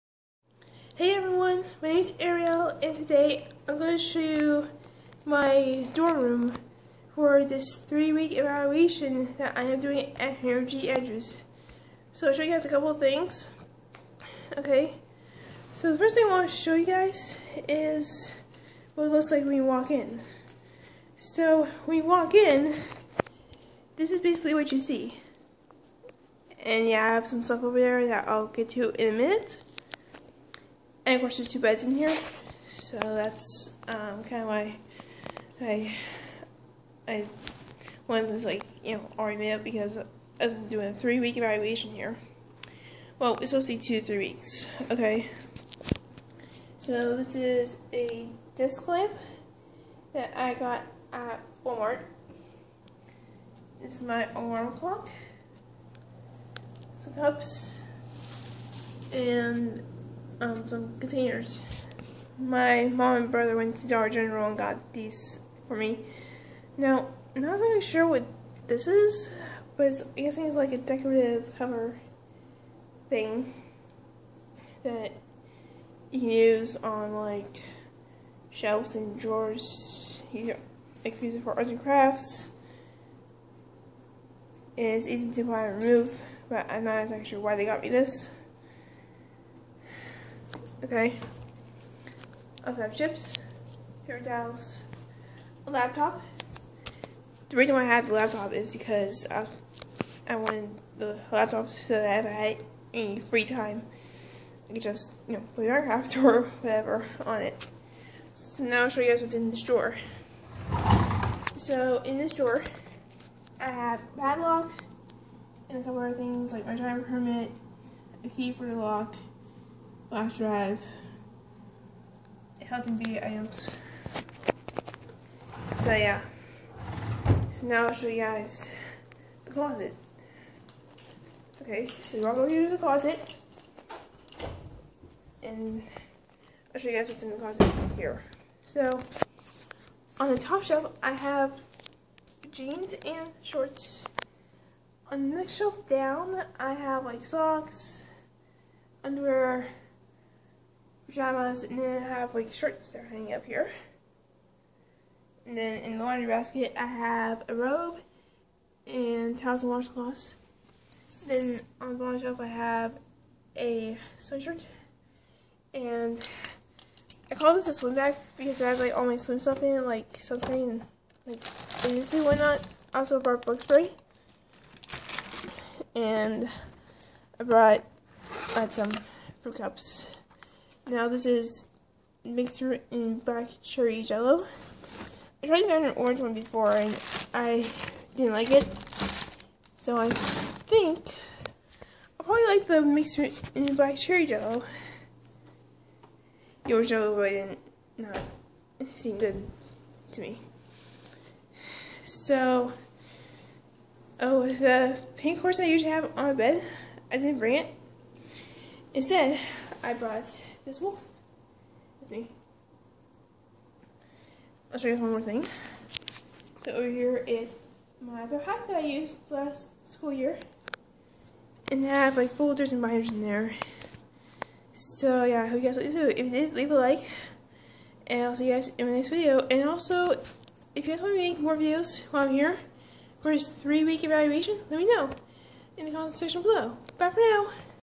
Room tour at Hiram G. Andrews center for the three week evaluation.
I set up all of the things in here. My mom and brother got some things at dollar general which is down the hill from the college.